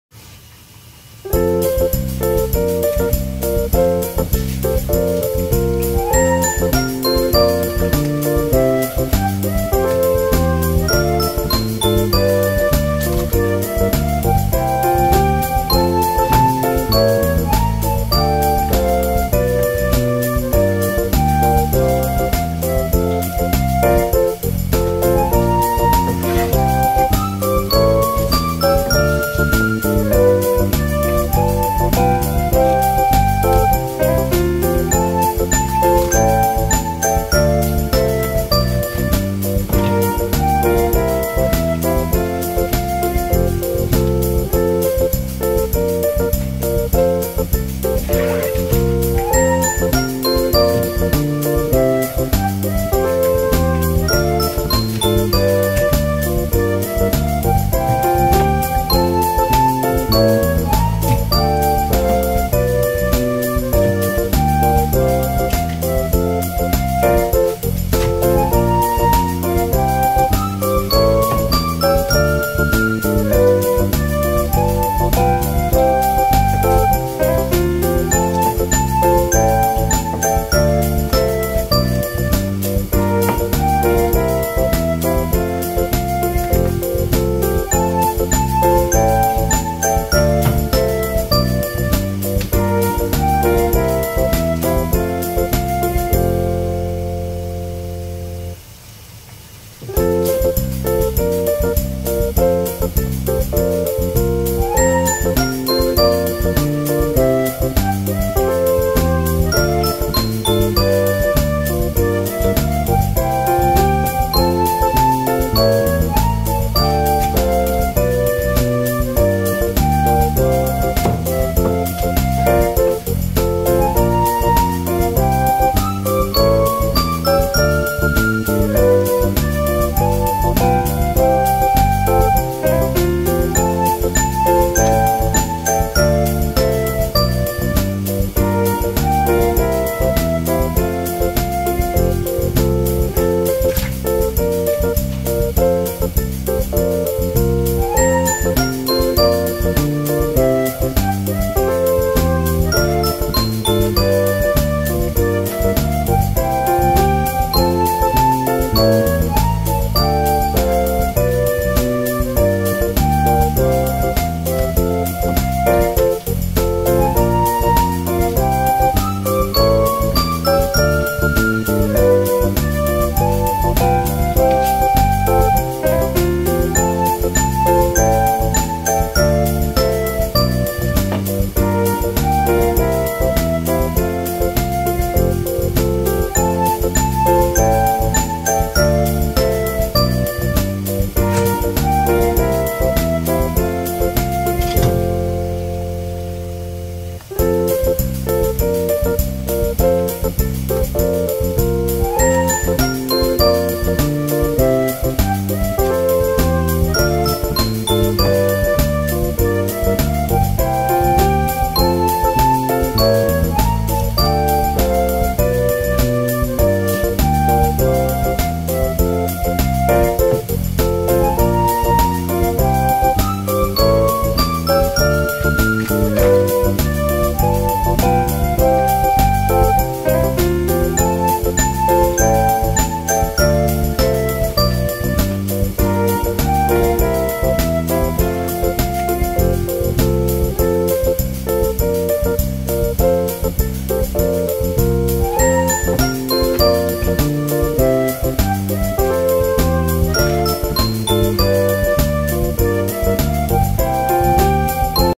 [魚の捌き方]スズキ三枚おろし！！
スズキをお刺身用三枚おろしにしました！
魚はいろんな捌き方が有るので少しでも参考になれば幸いです
保健所によると流水には殺菌効果が有るとの事なので水は流しっぱなしです